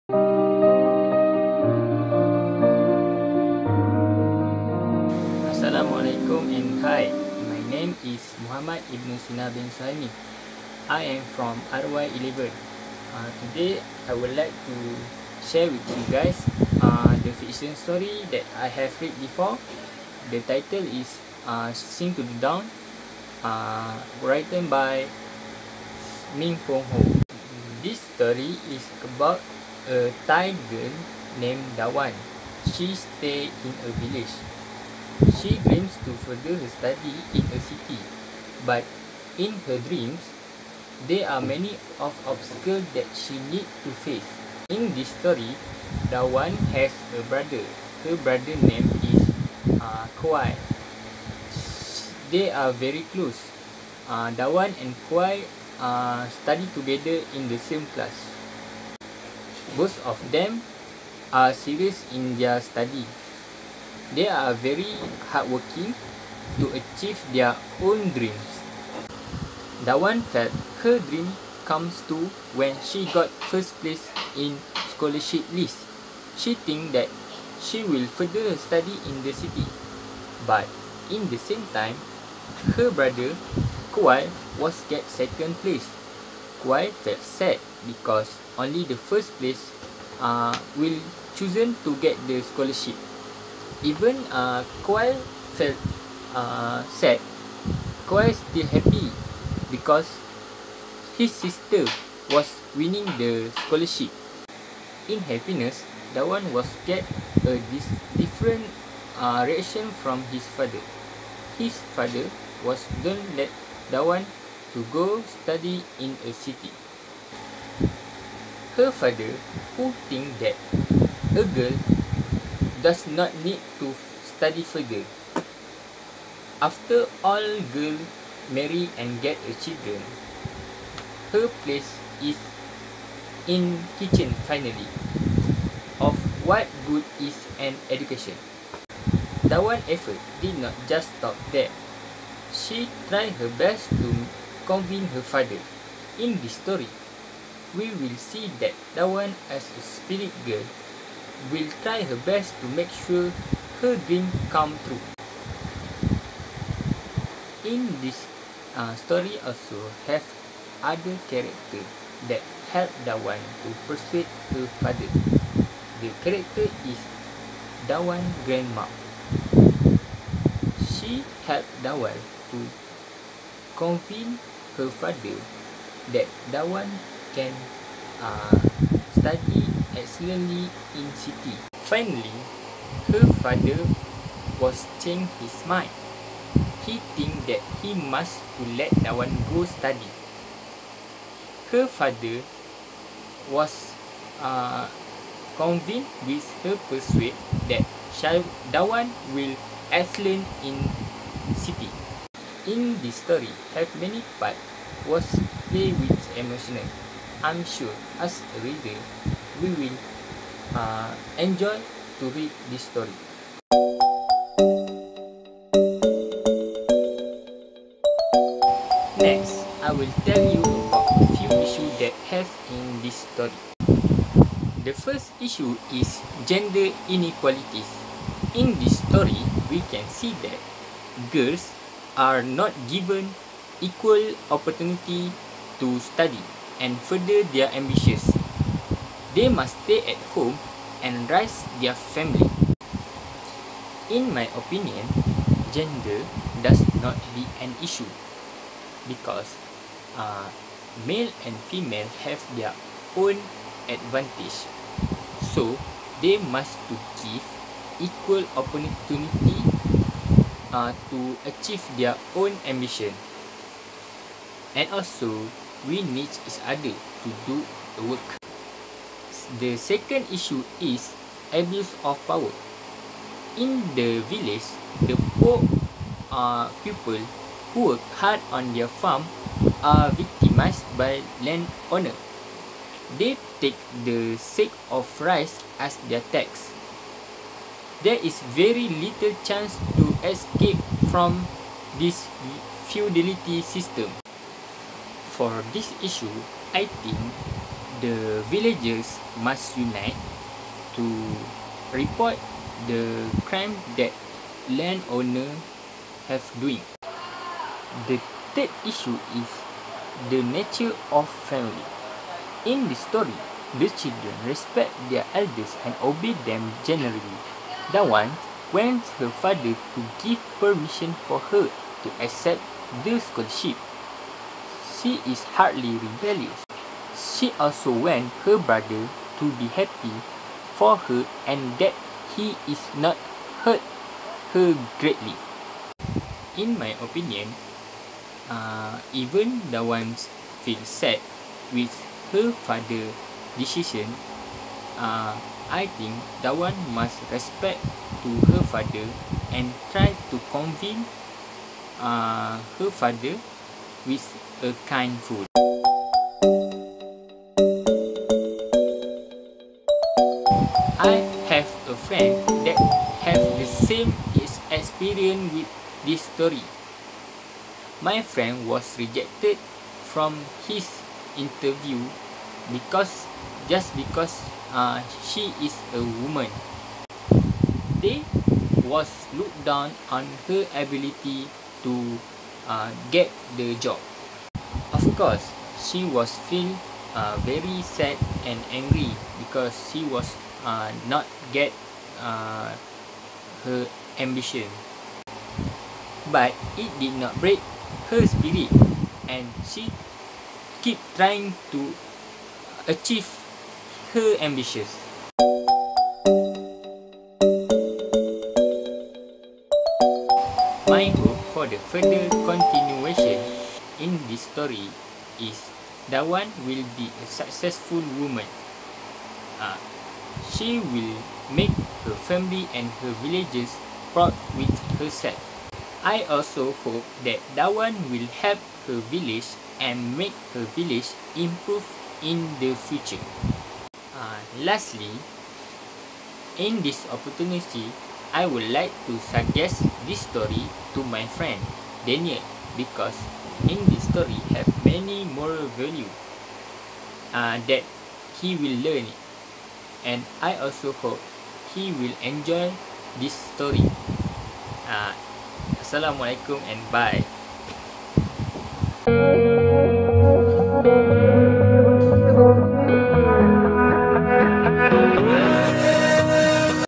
Short story (Sing to The Dawn)
Reading assignment
Subject English For General Communication
Present by Muhammad Ibnu Sina bin Suhaimi (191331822)

Title: Sing to The Dawn
Author: Minfong Ho
Published: 1975